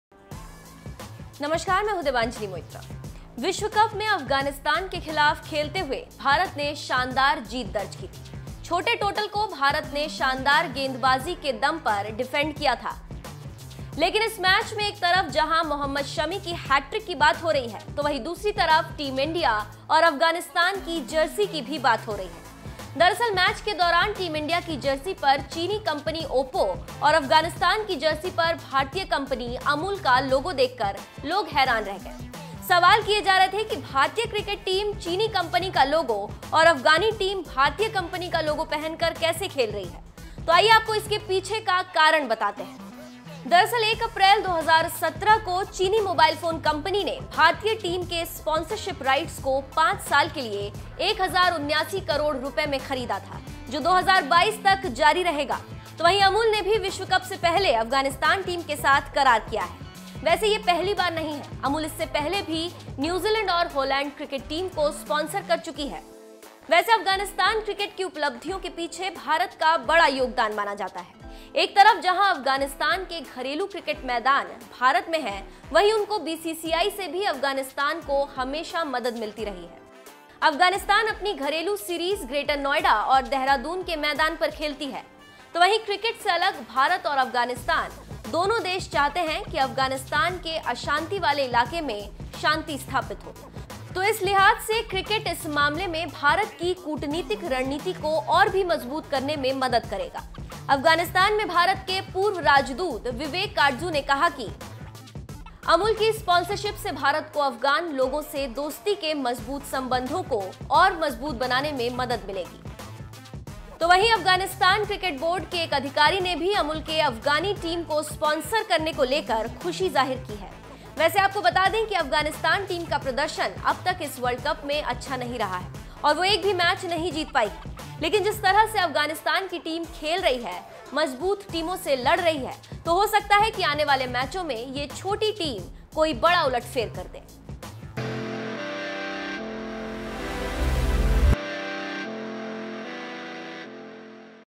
India की जर्सी पर ओप्पो तो Afghanistan की जर्सी पर Amul का लोगो क्यों?
विश्व कप में भारत की टीम की जर्सी पर चीनी कंपनी तो अफगानिस्तान की जर्सी पर अमूल का लोगो देखकर लोगों ने पूछे सवाल। जानिए क्या है इसके पीछे की वजह।